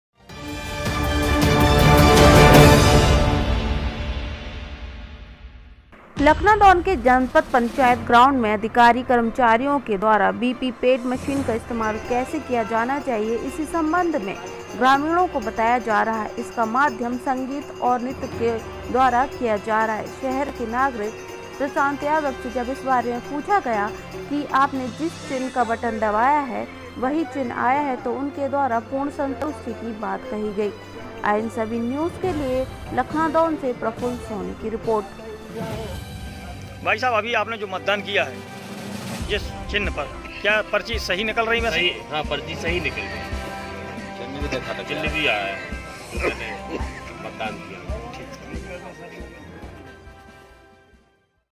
अधिकारी कर्मचारियों द्वारा बी बी पेड वोटिंग मशीन की दे रहे संगीत द्वारा समझाइश
लखनादौन के अधिकारी कर्मचारियों द्वारा बी बी पेड वोटिंग मशीन की दे रहे संगीत द्वारा समझाइश

//लखनादौन के जनपद पंचायत ग्राउंड में अधिकारी कर्मचारियों के द्वारा बी बी पेट मशीन का इस्तेमाल कैसे किया जाना चाहिए इसी संबंध में ग्रामीणों को बताया जा रहा है इसका माध्यम संगीत और नृत्य को रखा गया है
शहर के नागरिक प्रशांत यादव से पूछा गया आपने जिस चिन्ह का बटन दबाया क्या वहीं आया उनके द्वारा पूर्ण संतुष्टि की बात कही गई
बाइट करता प्रशांत यादव